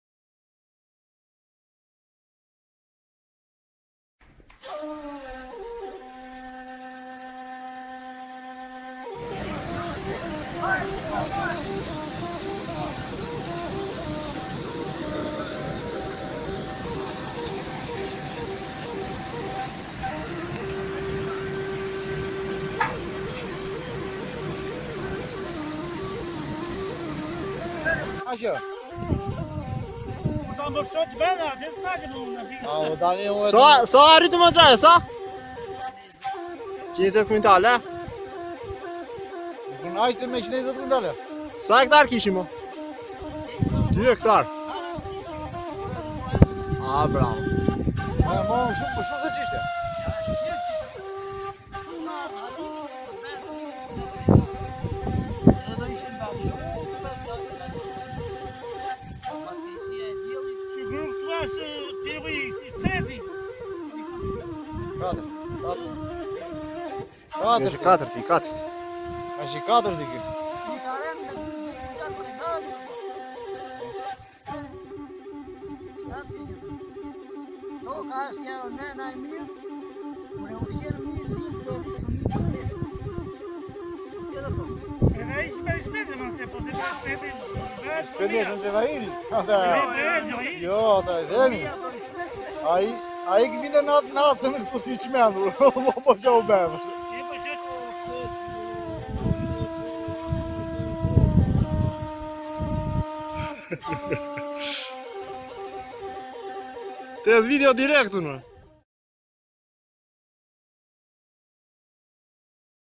REHOVE BERAT -SHIRRJA E GRURIT ME AUTOKOMBANJ 🌾🌾 QINDRA TON GRURE
SHIRRJA E GRURIT 🌾 REHOVE BERAT *XHUNOVE *
PRODHIMI I GRURIT VITI KORRIK  2017  PA FUND ...  super fshatra puntore super prodhimtari ..